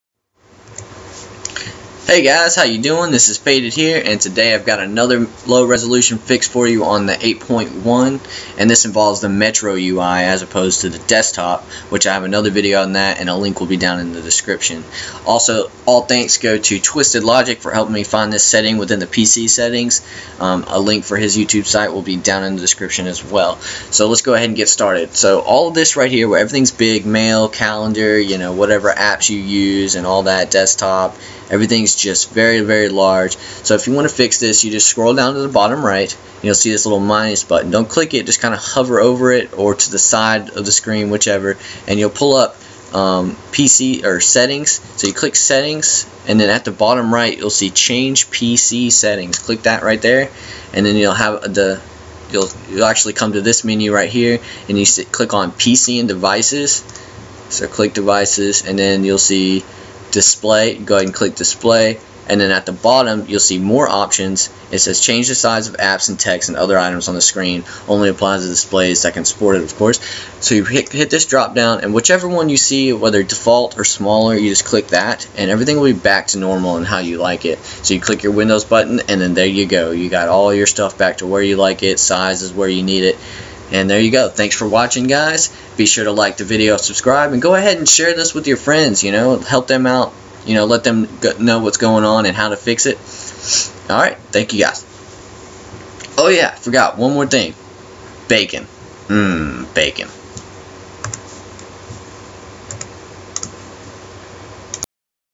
Windows 8.1 Low Metro App Resolution Fix
Windows 8.1 Upgrade changes the PC settings within the display section. This caused a lot of programs to run at a scaled down or otherwise degraded resolution. This is how you fix it!

Any questions just let me know!

AND
►Use this link before placing an order